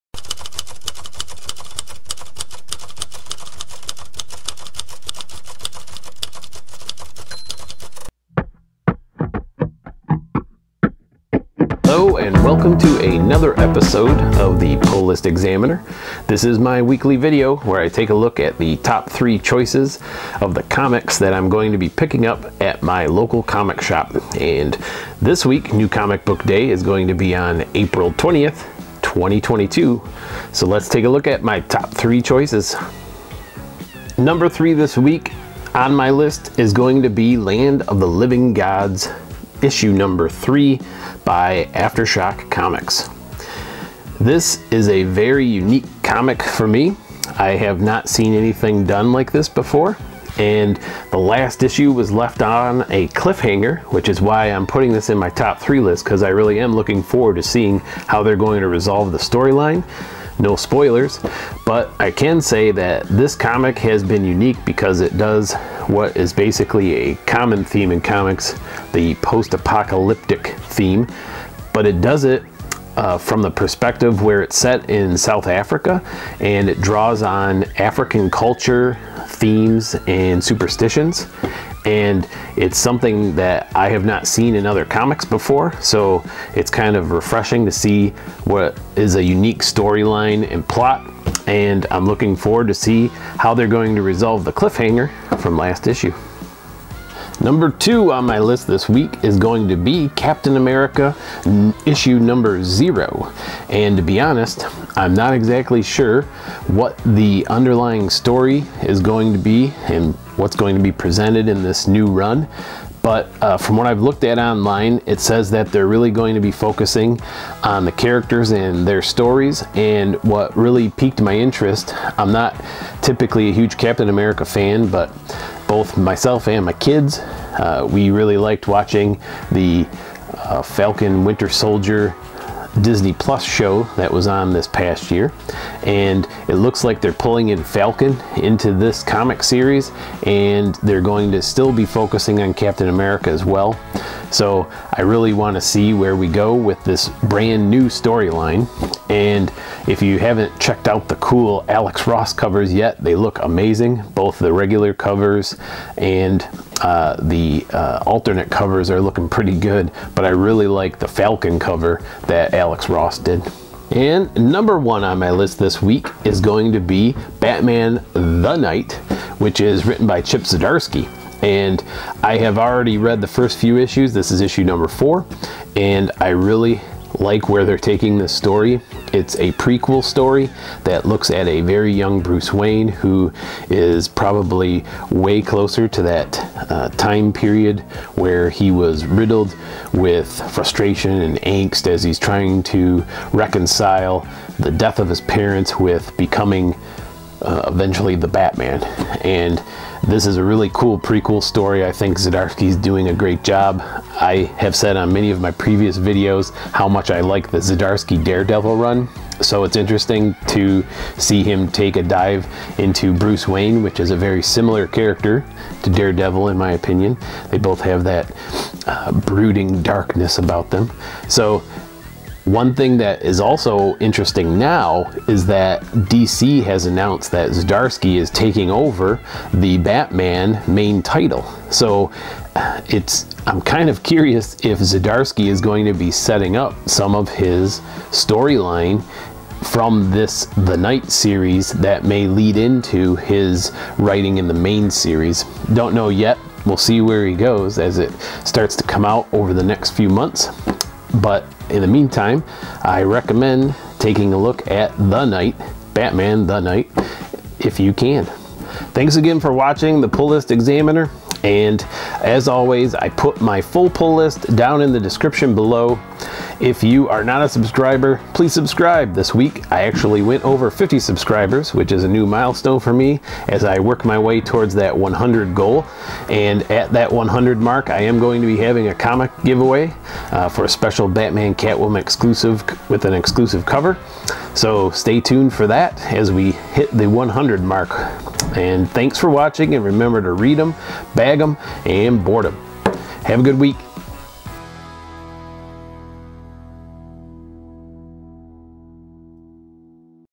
Top 3 Comic Book Picks [Weekly Pull List - April 20, 2022]
Hello, every weekend I post my top three choices from my weekly pull list for New Comic Book Day (NCBD) Wednesday.  This week NCBD is on Wednesday, April 20, 2022.  Please watch to hear why I am picking these as my top three choices.

My full pull list for this week looks like this:
Animal Castle 5
Batman/Superman Worlds Finest 2
Batman The Knight 4
Captain America 0
Catwoman Lonely City 3
Land of the Living Gods 3
Nightwing 91
Primos 3
Stranger Things Kamchatka 2
Wolvie 20